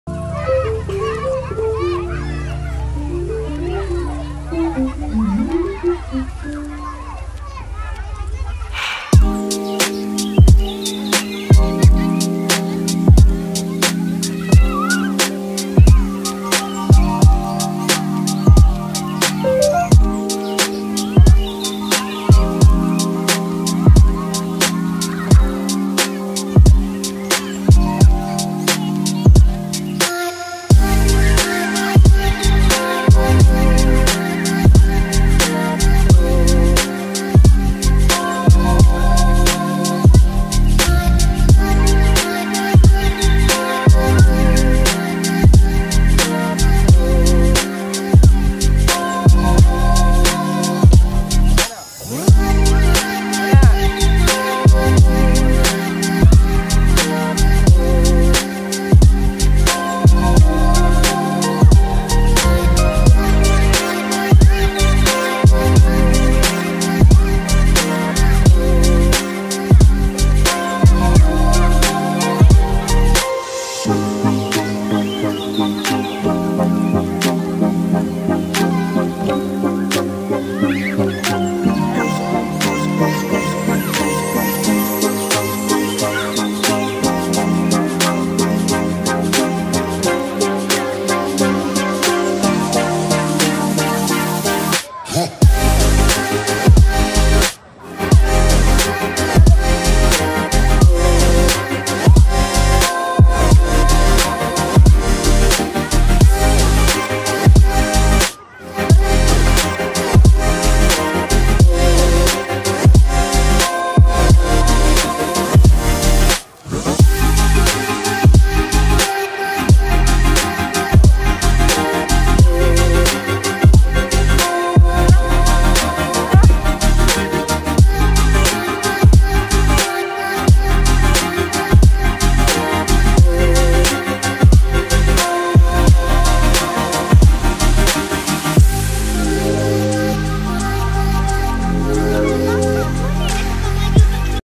Riadah di taman permainan kanak-kanak, taman tasik titiwangsa
dzil naufal enjoy dgn aktiviti di playground ni.